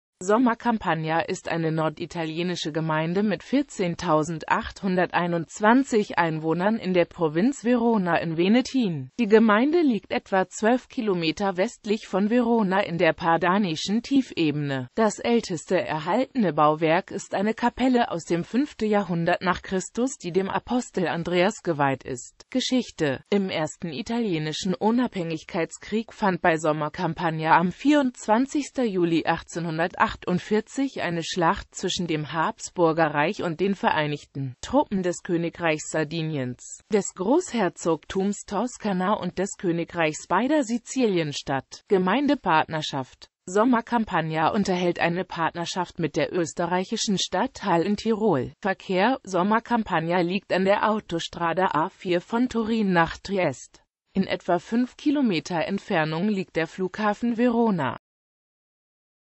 Sommacampagna
Vielen Dank für Ihre Unterstützung:

Sommacampagna ist eine norditalienische Gemeinde mit 14.821 Einwohnern in der Provinz Verona in Venetien.Die Gemeinde liegt etwa 12 Kilometer westlich von Verona in der padanischen Tiefebene.Das älteste erhaltene Bauwerk ist eine Kapelle aus dem 5.Jahrhundert nach Christus, die dem Apostel Andreas geweiht ist.

Bild-Copyright-Informationen
Urheber Info: concept: Rainer Zenz (2004) SVG and small adaptions: Huhsunqu and TM